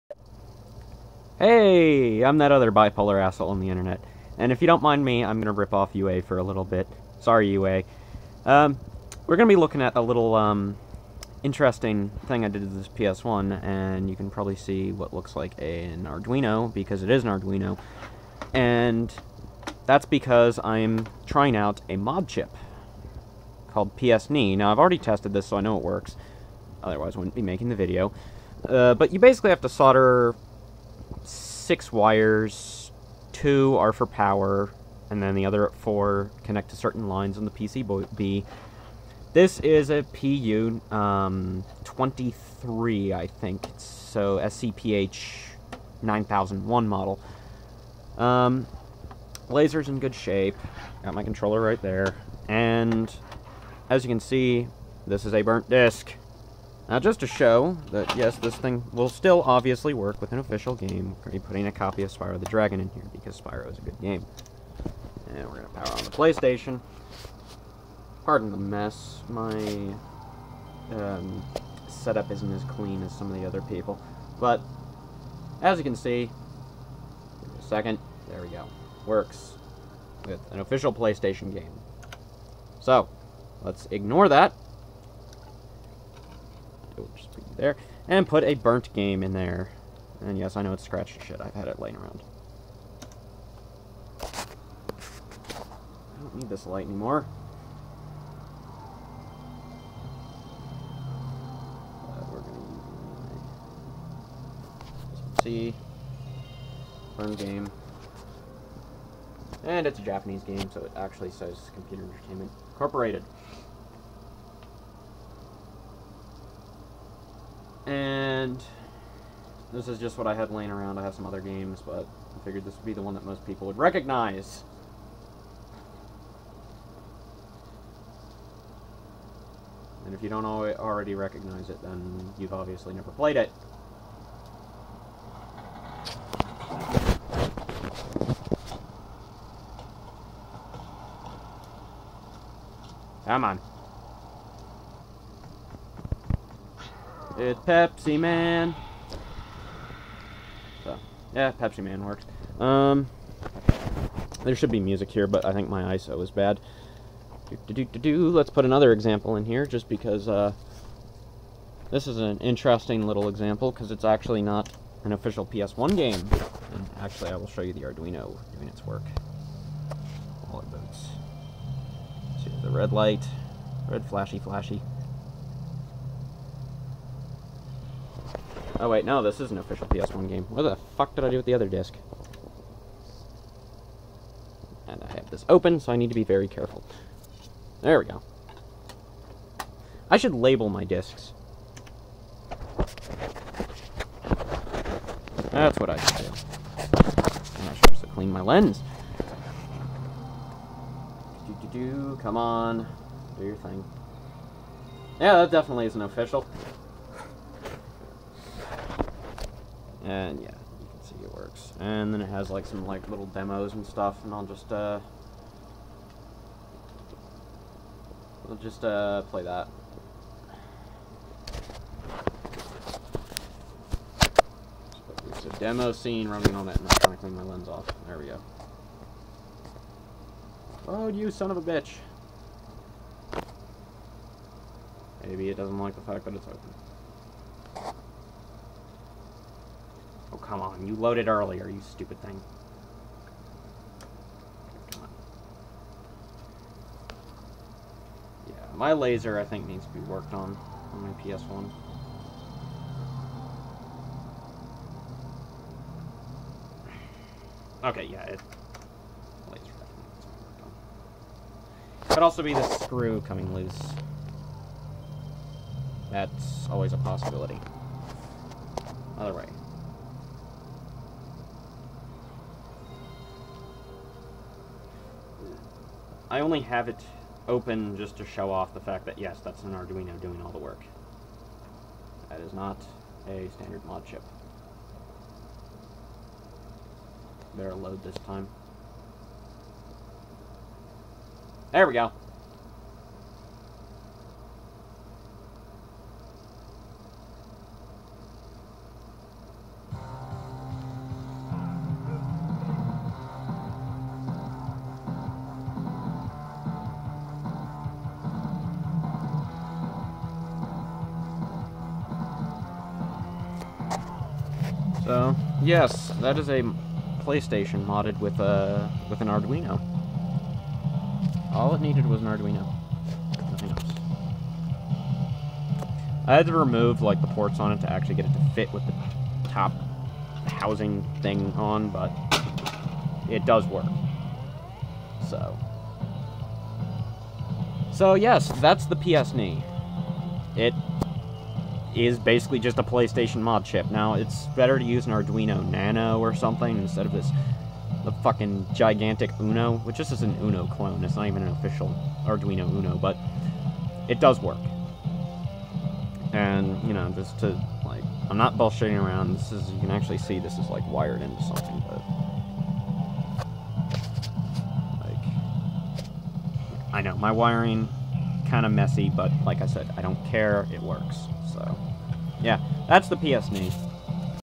Bootleg Game Lab: Arduino based Modchip for the PlayStation! (PsNee)
(ripping off UA with the title a bit but whatever)
Let's take a look at a modchip alternative for the PlayStation!